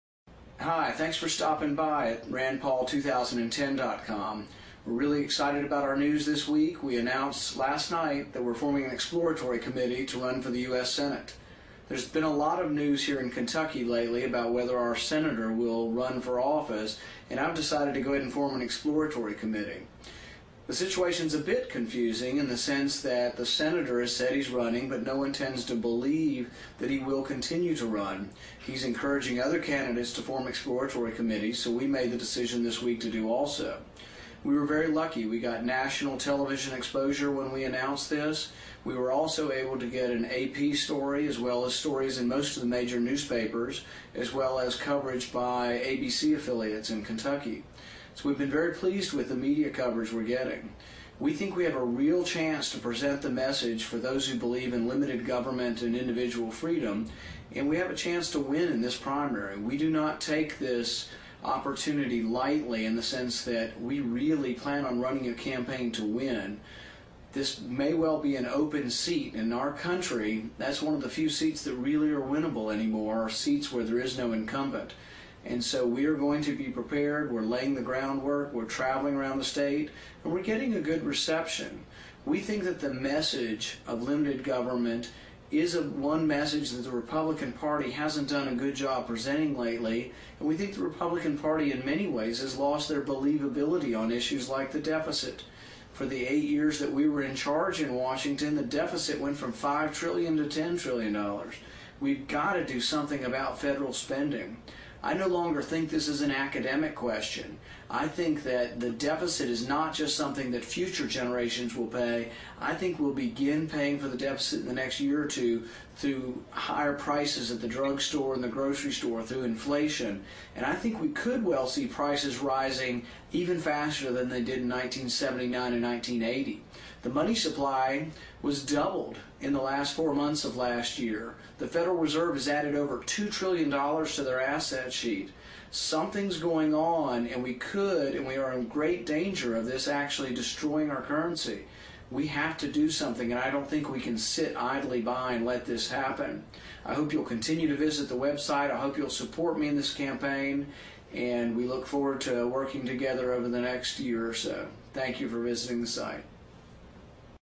YEAH!!! Go Rand!!!! - Rand Paul Welcomes You to his Website 5 15 09
Rand Paul is the third of five children. He grew up in a small town in Texas and attended Baylor University before leaving to go to Duke Medical School. Dr. Paul completed a General Surgery Internship at Georgia Baptist Medical Center in Atlanta and earned his ophthalmology residency at Duke University.
Dr. Paul is married to his loving wife, Kelley Ashby, of Russellville, KY. They have been married for 18 years and have three boys: Will 16, Duncan 13, and Robert 10.

Dr. Paul has owned his own business, performing eye surgery in Bowling Green, KY, for 18 years. He is also the founder of the Southern Kentucky Lions Eye Clinic, an organization that performs eye exams and surgery for less fortunate patients.

Additionally, Dr. Rand Paul founded the Kentucky Taxpayers United (KTU) in 1994. KTU rates the state legislatures tax honesty and helps promote the Americans for Tax Reform taxpayer pledge, to oppose any and all marginal income tax increases and any reduction in tax deductions.

Dr. Rand Paul delivered his first public speech in a debate with Senator Phil Gramm at the age of 21 in San Antonio.   For his father's Presidential campaign, Dr. Rand Paul traveled throughout ten different states.  In December of 2007, he spoke to thousands of supporters at the historic Fanueil Hall in Boston on the day his fathers campaign set the record for most online donations in a single day.

Rand Paul is a career doctor, not a politician.  His entrance into politics is indicative of his lifes work: a desire to diagnose problems, and provide practical solutions.